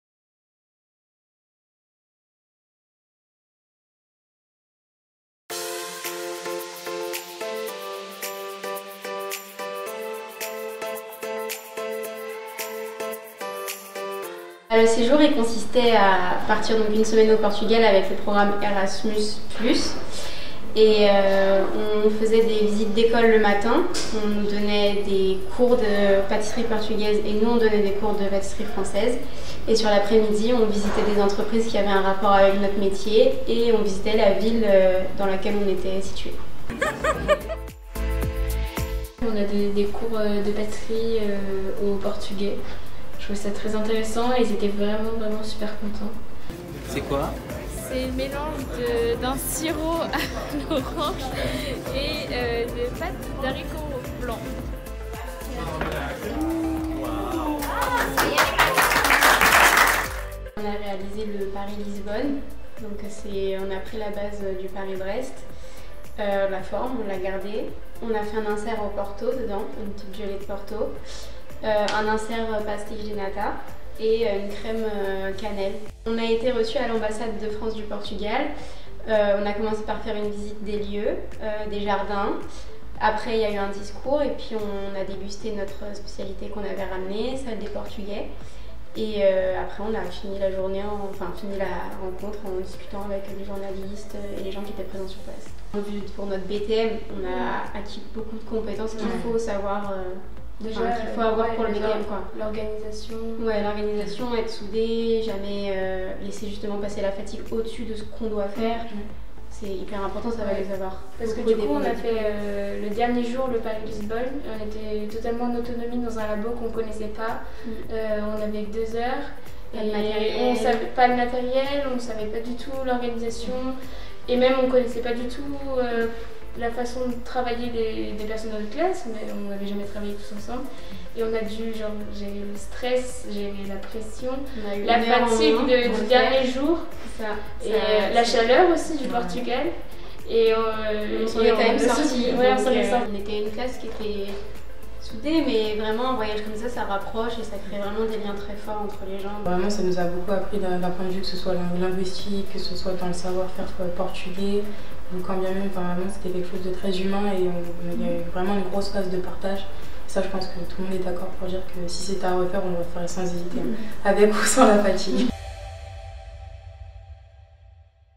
SEJOUR PEDAGOGIQUE D’APPRENTIS DE BTM A LISBONNE
Revivez comme si vous y étiez le séjour de neuf apprentis de BTM pâtisserie à Lisbonne en le 16 et le 22 mai. Un expérience professionnelle et humaine passionnante durant laquelle ils ont travaillé en immersion au sein de prestigieuses écoles culinaires du réseau Turismo de Portugal et découvert par la même occasion les trésors culinaires et culturels de la capitale portugaise.